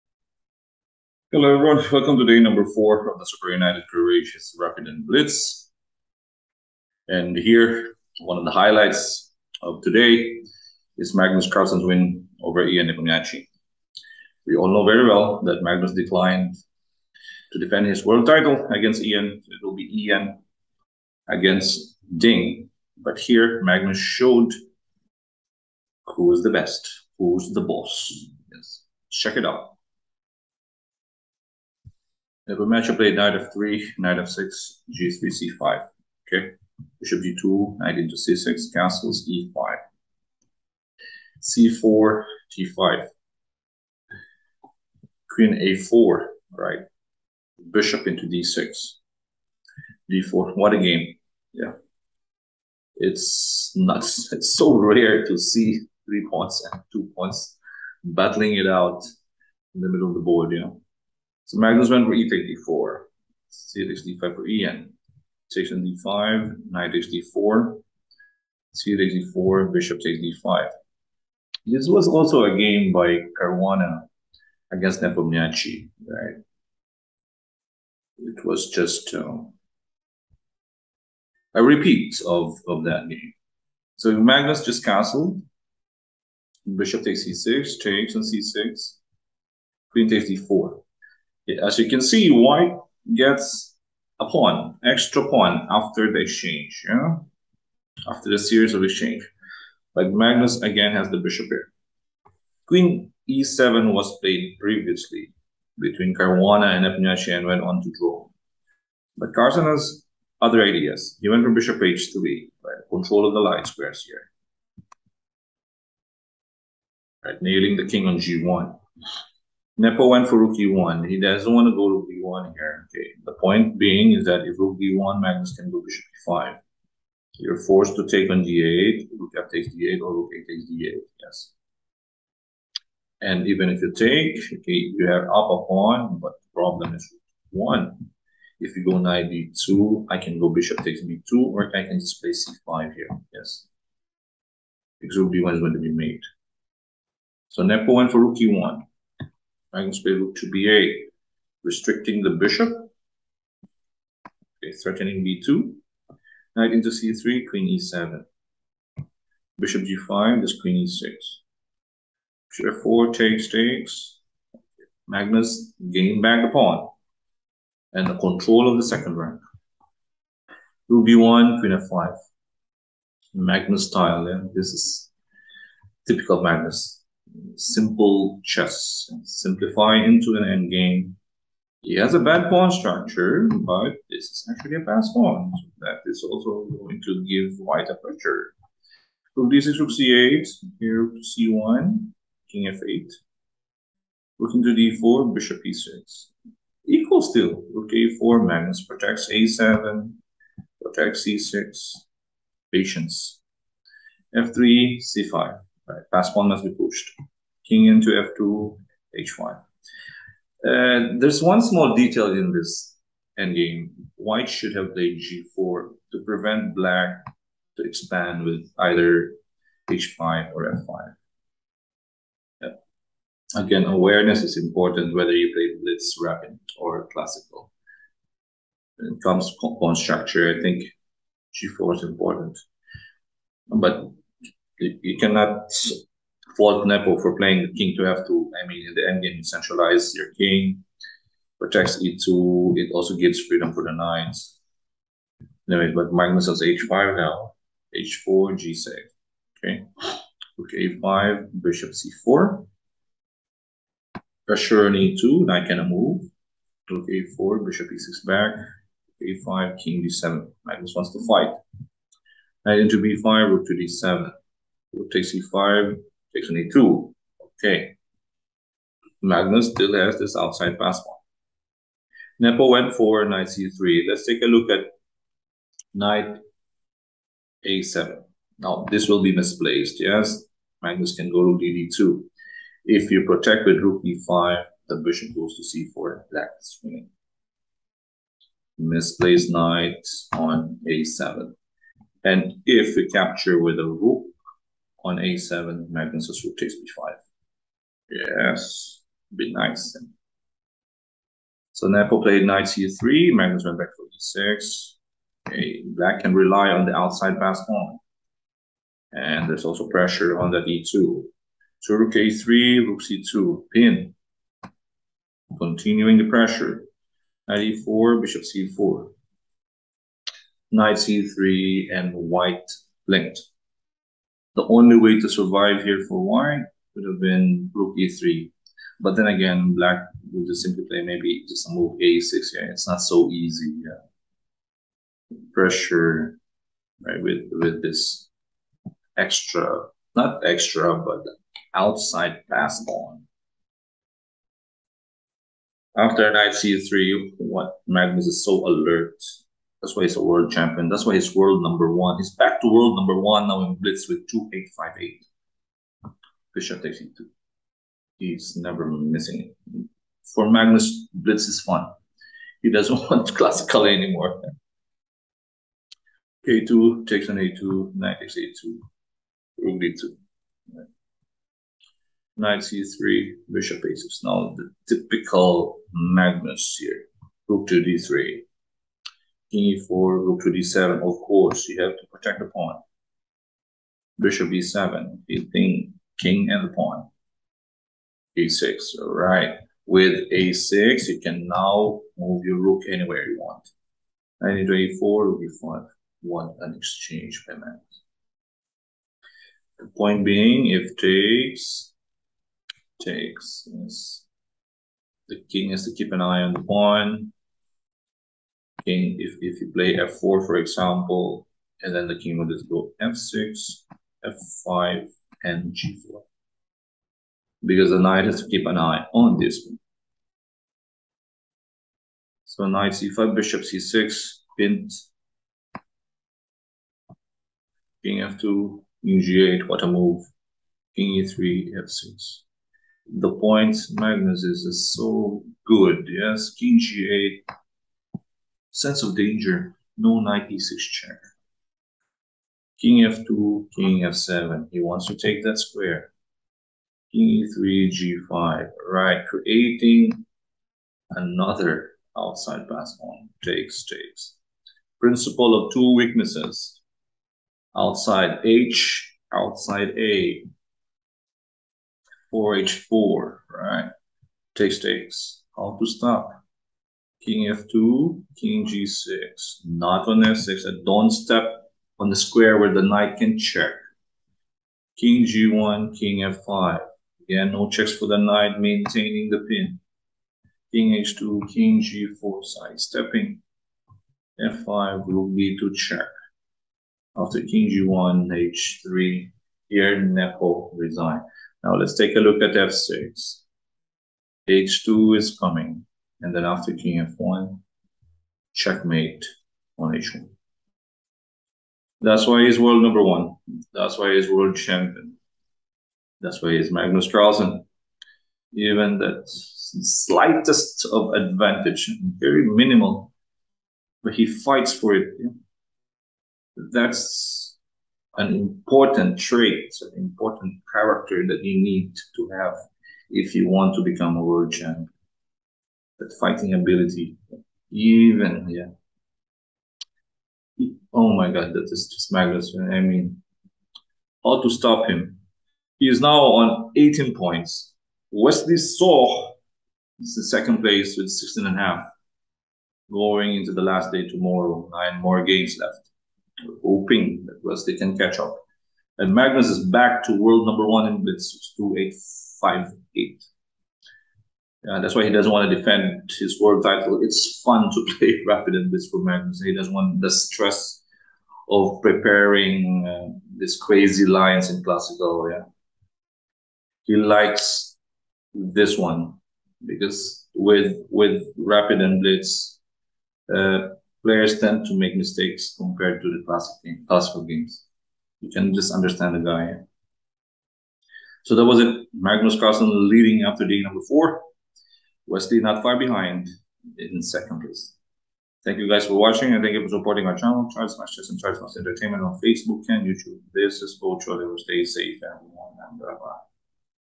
Magnus Carlsen on beast mode, back to World No 1 in Blitz with 2858!
Super United Croatia Rapid & Blitz